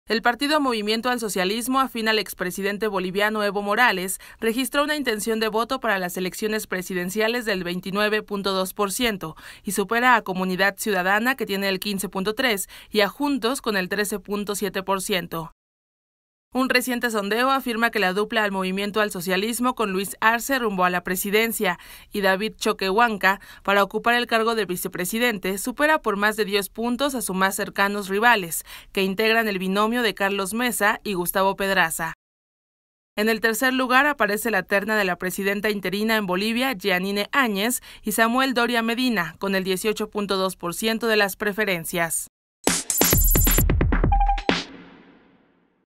MAS lidera preferencias rumbo a comicios presidenciales de Bolivia
El partido Movimiento Al Socialismo, afín al expresidente  Boliviano Evo Morales, registró una intención de voto para las elecciones presidenciales del 29.2 por ciento, y supera a Comunidad Ciudadana que tiene el 15.3, y a Juntos con el 13.7 por ciento.